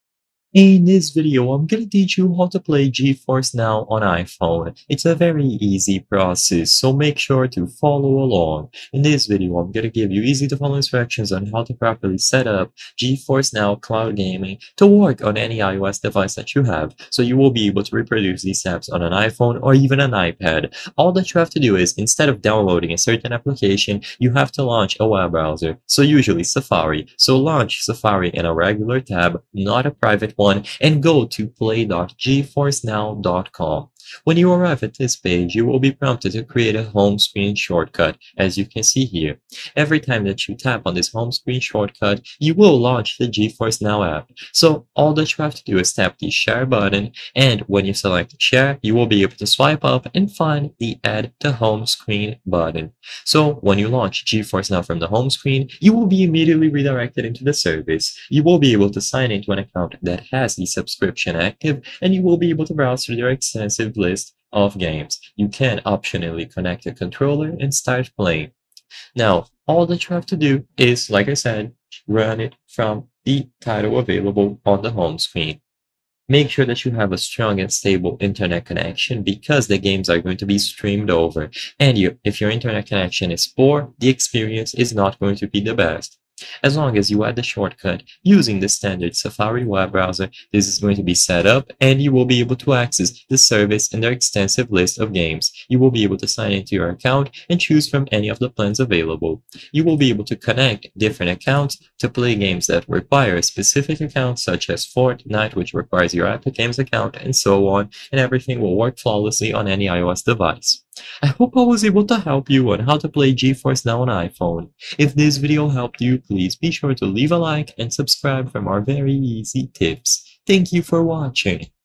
How To Play Geforce Now on iPhone 2025 - (UPDATED!)
I will solve your doubts about how to play geforce now on iphone, and whether or not it is possible to do this.

I hope I have helped you with the video!